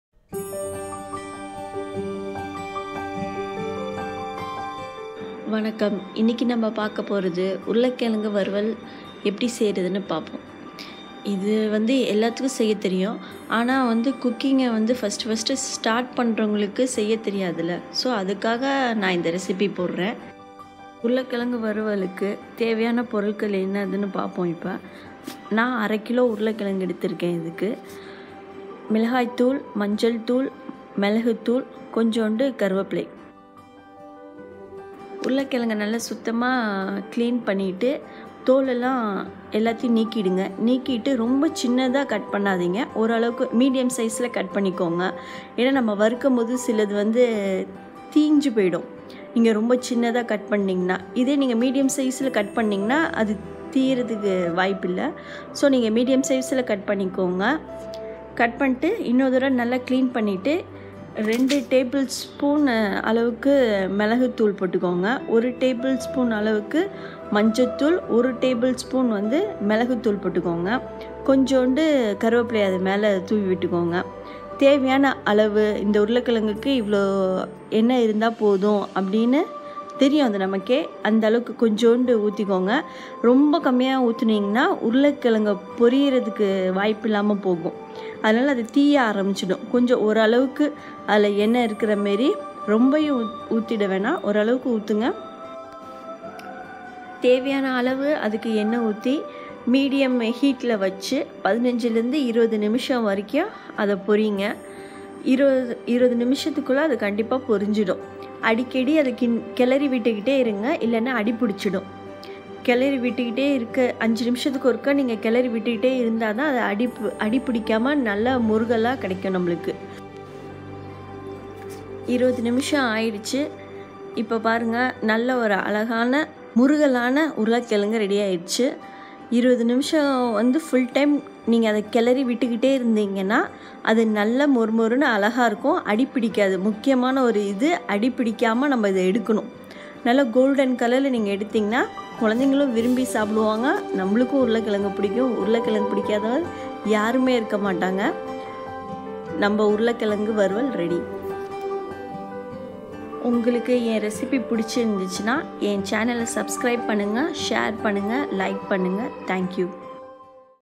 Potato Fry in Tamil Recipe | Urulai Kizhangu Poriyal/Varuval | உருளை கிழங்கு வறுவல்
In this video I will see how to make Potato fry in Tamil. Potato Poriyal/Varuval  / Urulai Kizhangu Poriyal/Varuval  in a simple to make recipe. This is a easy and quick recipe for kids lunch boxes and to make something quick for lunch.

Please try this simple recipe and post your feedback in the comments below. Happy Cooking!

Ingredients:
1. Potato - 4
2. Chilli Powder - 1 spoon (depending upon level of spicyness required)
3. Milagu powder - 1/2 teaspoon
4. Manjal Powder - 1/4 teaspoon
5. Salt - To Taste
6. Oil - For Frying
7. Curry Leaves - Little bit